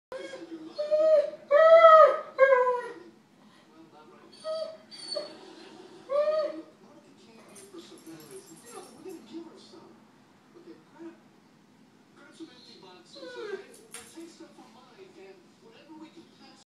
Frustrated Rudo
Rudo whines to play